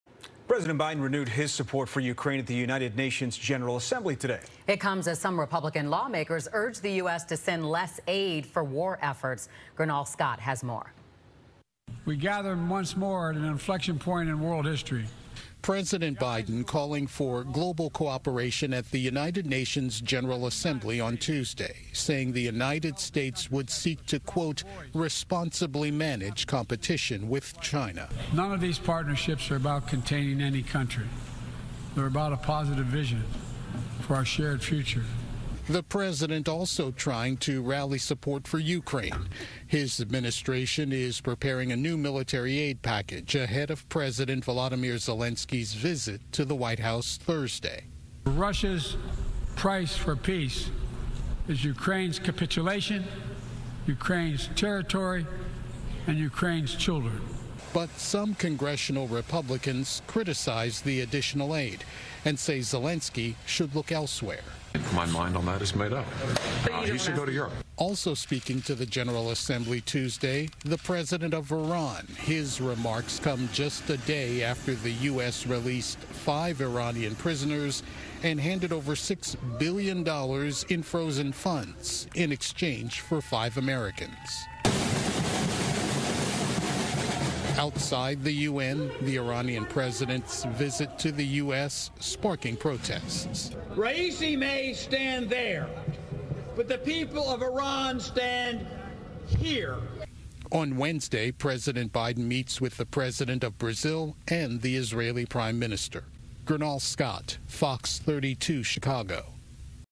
President Joe Biden addresses United Nations
President Biden renewed his support for Ukraine at the United Nations General Assembly Tuesday as some Republican lawmakers urged the U.S. to send less aid for war efforts.